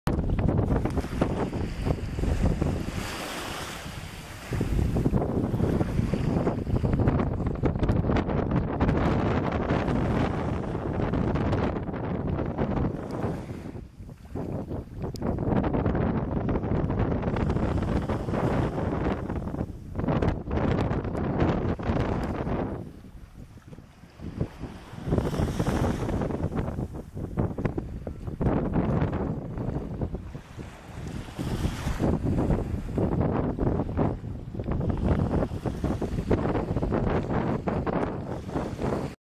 Eagle Beach, Aruba
beautiful Eagle Beach on One Happy Island - in my opinion the best beach on the island. It's less crowded than Baby Beach.